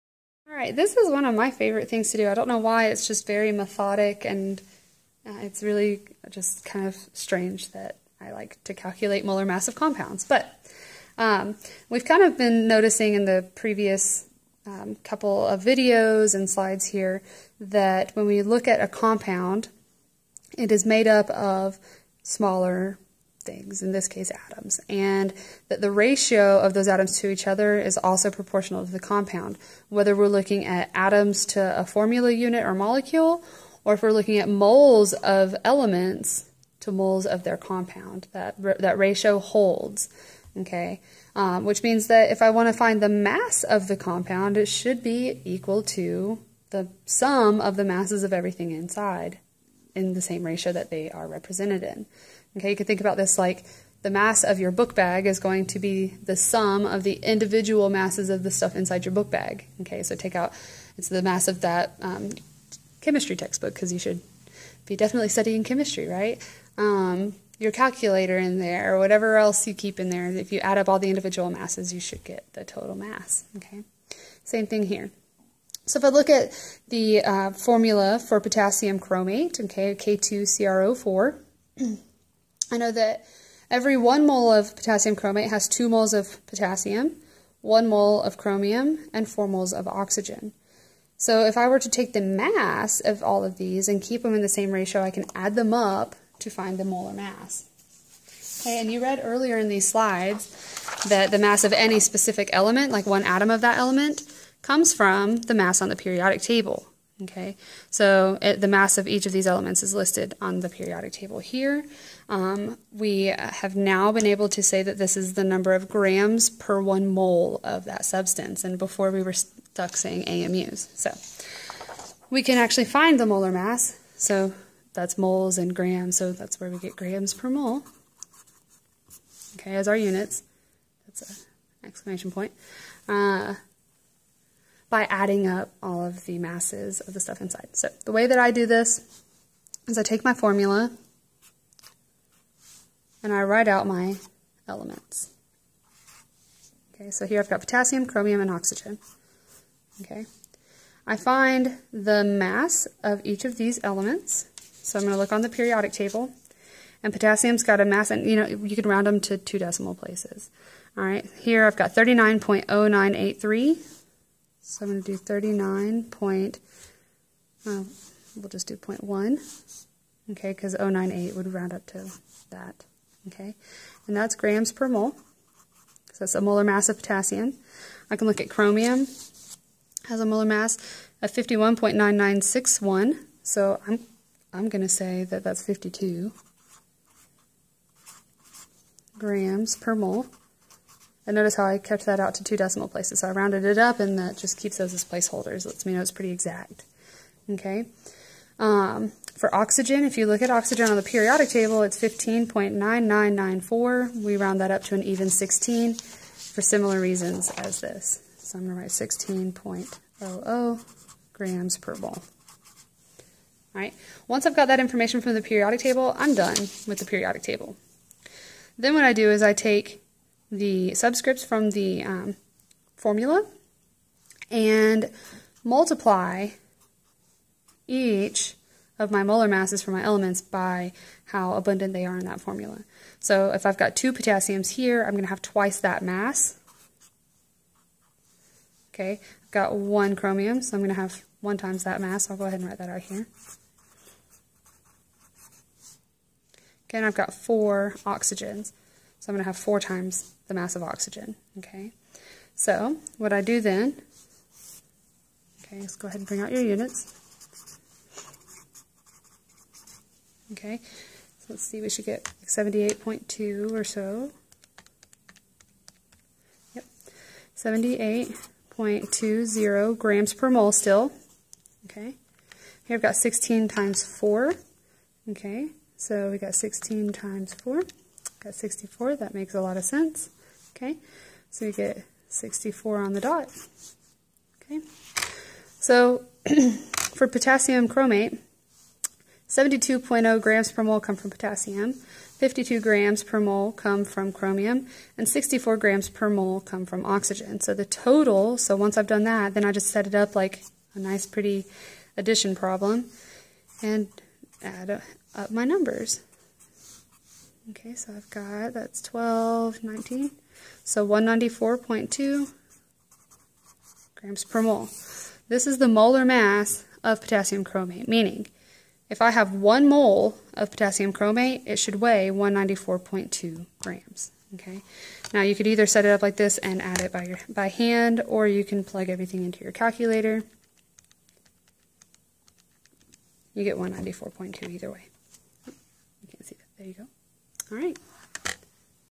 MOLAR MASS - (slide 13)
Covers how to calculate molar mass of a compound, using potassium chromate as an example.